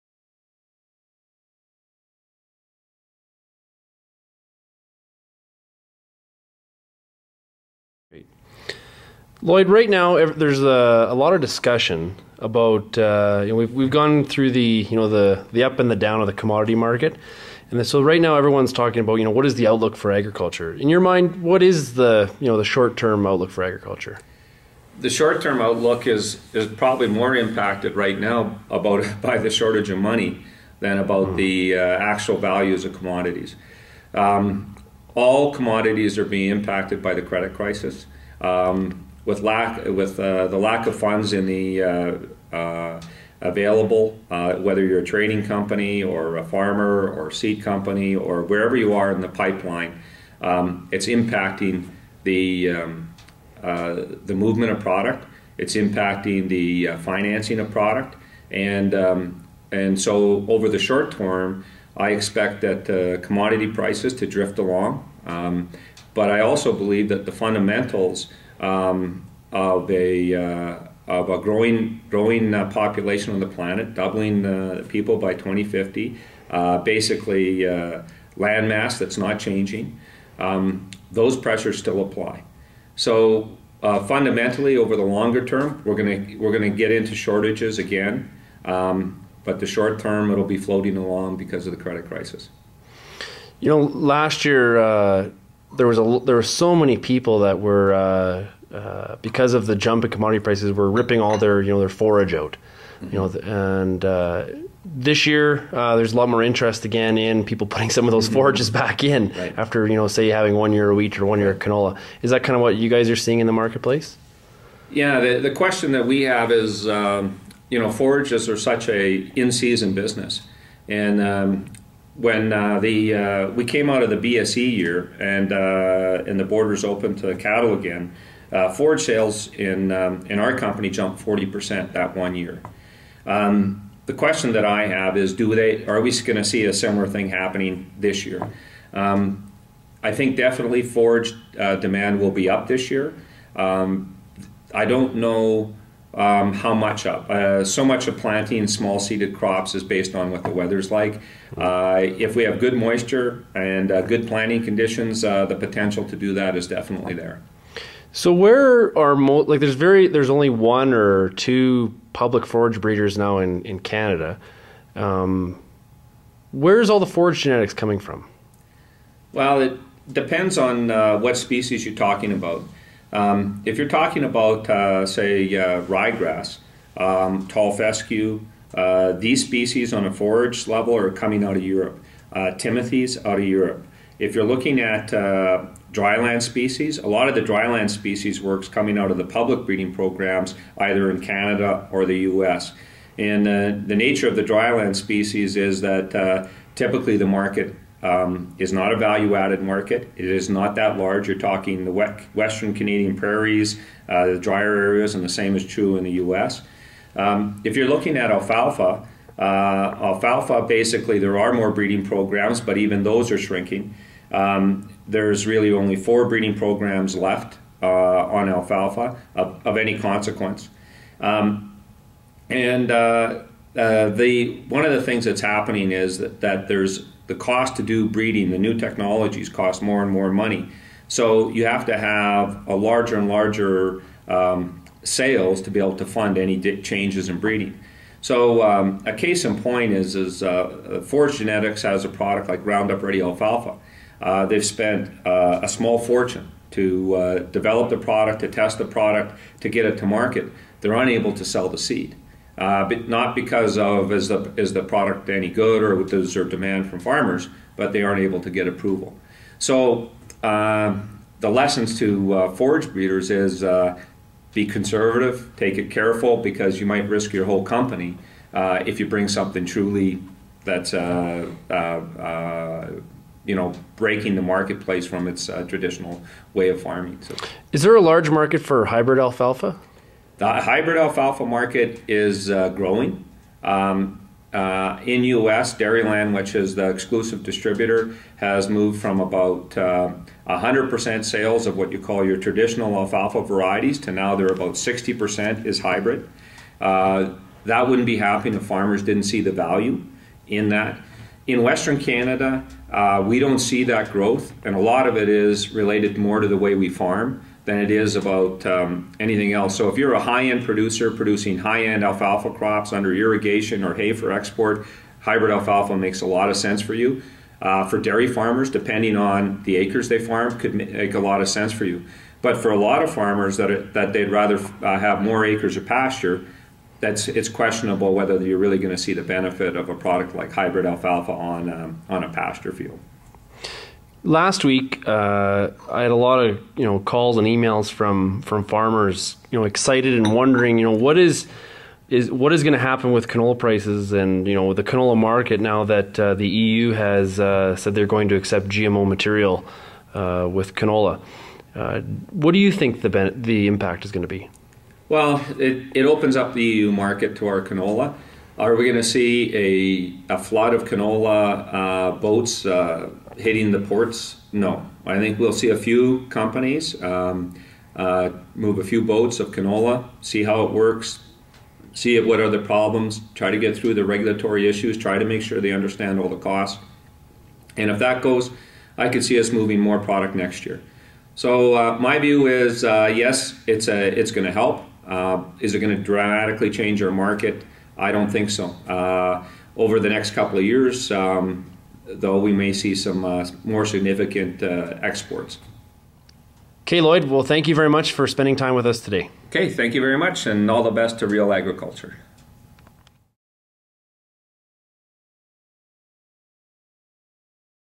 Part 2 - Lloyd Dyck CEO of Brett Young - commodities, forage industry and GMO's
Lloyd Dyck discusses how the volatile commodity market is affecting the access to credit, the forage business and how the EU accepting GM canola will affect Canadian agriculture.